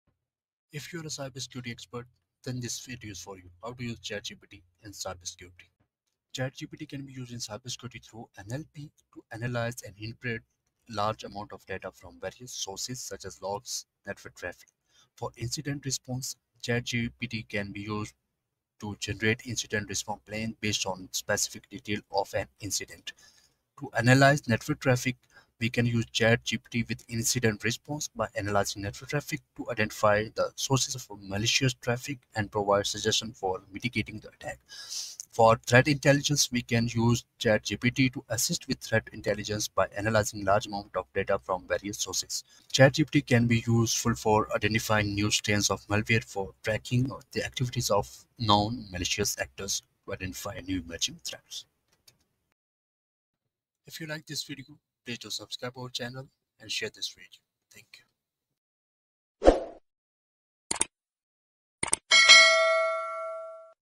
ChatGPT For Cybersecurity | OpenAI | Productivity
ChatGPT is an AI-driven chatbot launched by OpenAI in November 2022.
It is trained using Reinforcement Learning from Human Feedback (RLHF).
It is built on top of OpenAI's GPT-3.5 family of large language models and is fine-tuned with both supervised and reinforcement learning techniques.